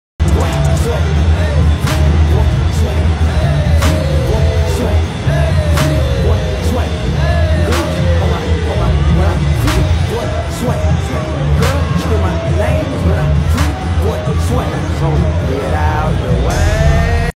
Girls Kept Kawhi’s SWEATY Towel 😅
Los Angeles Clippers star Kawhi Leonard was holding a towel as he entered the court and randomly threw it to a fan which made the fan confused but decided to keep it.